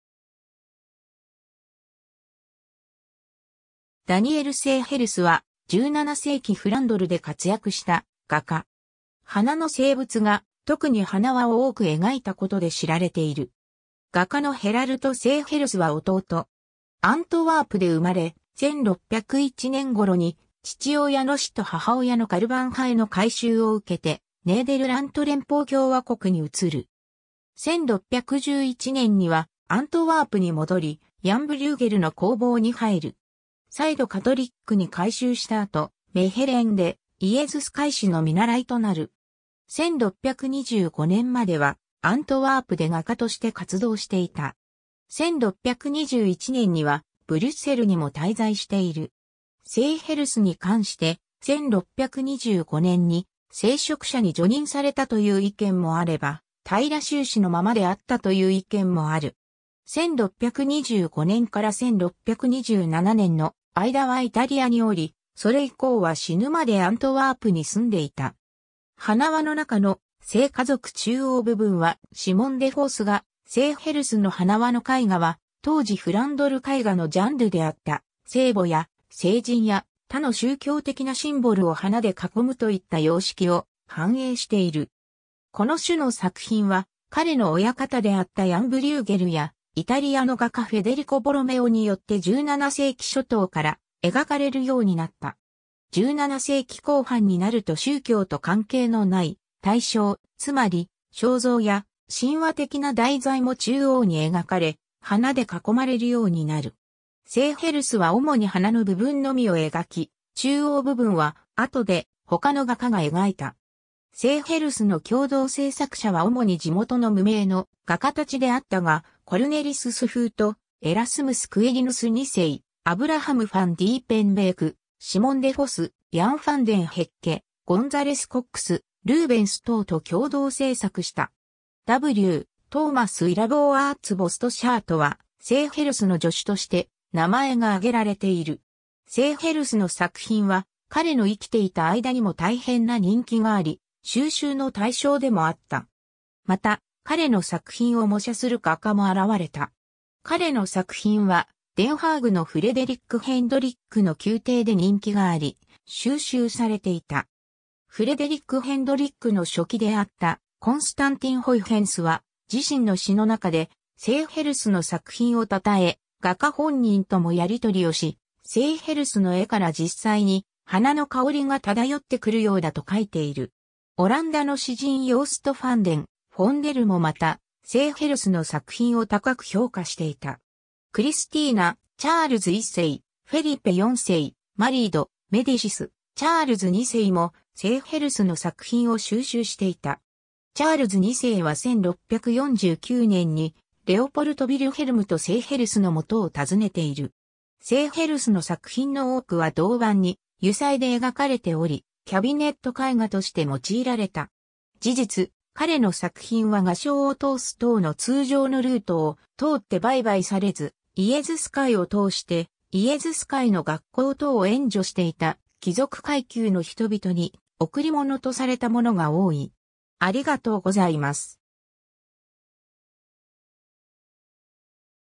ダニエル・セーヘルス
ダニエル・セーヘルス (Daniel Seghers, 1590年12月3日 – 1661年11月2日) は、17世紀フランドルで活躍した画家。
花の静物画、特に花環を多く描いたことで知られている。
画家のヘラルト・セーヘルスは弟。
 アントワープで生まれ、1601年頃に父親の死と母親のカルヴァン派への改宗を受けてネーデルラント連邦共和国に移る。
1611年にはアントワープに戻り、 ヤン・ブリューゲルの工房に入る。
再度カトリックに改宗した後、メヘレンでイエズス会士の見習いとなる。
1625年まではアントワープで画家として活動していた。
1621年にはブリュッセルにも滞在している。
セーヘルスに関して、1625年に聖職者に叙任されたという意見もあれば、平修士のままであったという意見もある。
 1625年から1627年の間はイタリアにおり、それ以降は死ぬまでアントワープに住んでいた。
 『花環の中の聖家族』(1644) 中央部分はシモン・デ・フォス画 セーヘルスの花環の絵画は、当時フランドル絵画のジャンルであった、聖母や聖人や他の宗教的なシンボルを花で囲むといった様式を反映している。
この種の作品は彼の親方であったヤン・ブリューゲルや、イタリアの画家フェデリコ・ボロメオによって17世紀初頭から描かれるようになった。
17世紀後半になると宗教と関係のない対象、つまり肖像や神話的な題材も中央に描かれ、花で囲まれるようになる。
 セーヘルスは主に花の部分（開花しようとしているバラやチューリップ）のみを描き、中央部分は後で他の画家が描いた。
セーヘルスの共同制作者は主に地元の無名の画家達であったが、コルネリス・スフート、エラスムス・クエリヌス2世、アブラハム・ファン・ ディーペンベーク、シモン・デ・フォス、ヤン・ファン・デン・ヘッケ、ゴンザレス・コックス、ルーベンス等と共同制作した。
 w:Thomas Willeboirts Bosschaertはセーヘルスの助手として名前が挙げられている。
 セーヘルスの作品は彼の生きていた間にも大変な人気があり、収集の対象でもあった。
また、彼の作品を模写する画家も現れた。
 彼の作品はデン・ハーグのフレデリック・ヘンドリック (オラニエ公) の宮廷で人気があり、収集されていた。
フレデリック・ヘンドリックの書記であったコンスタンティン・ホイヘンスは自身の詩の中でセーヘルスの作品を称え、画家本人ともやり取りをし、セーヘルスの絵から実際に花の香りが漂ってくるようだと書いている。
 オランダの詩人ヨースト・ファン・デン・フォンデルもまたセーヘルスの作品を高く評価していた。
クリスティーナ (スウェーデン女王)、チャールズ1世 (イングランド王)、フェリペ4世 (スペイン王)、マリー・ド・メディシス、チャールズ2世 (イングランド王)もセーヘルスの作品を収集していた。
チャールズ2世は1649年にレオポルト・ヴィルヘルムとセーヘルスの元を訪ねている。
 セーヘルスの作品の多くは銅板に油彩で描かれており、キャビネット絵画として用いられた。
事実、彼の作品は画商を通す等の通常のルートを通って売買されず、イエズス会を通して、イエズス会の学校等を援助していた貴族階級の人々に贈り物とされたものが多い。